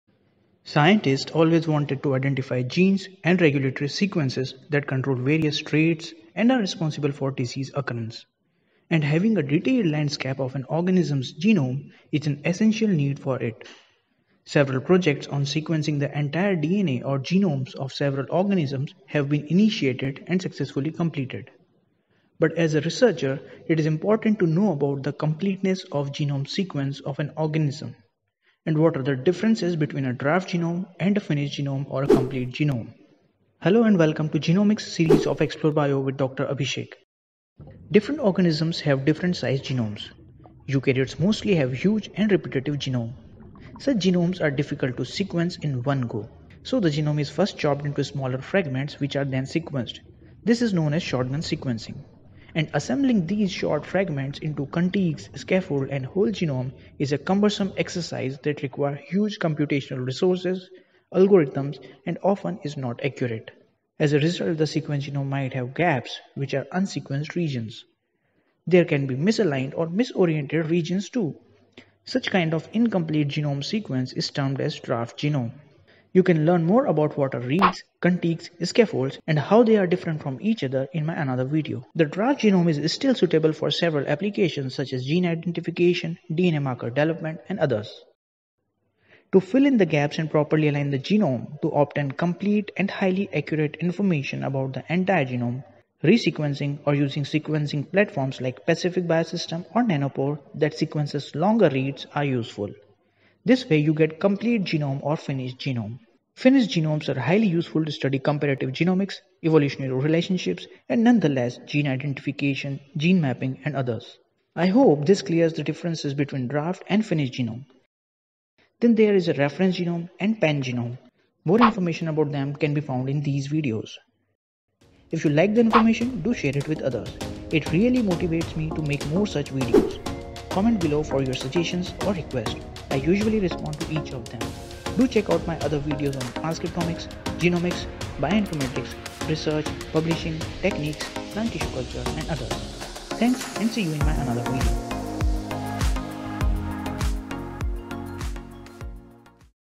What is draft and a finished genome? What is the difference?
Learn about the differences between a draft genome and a reference genome in this short video.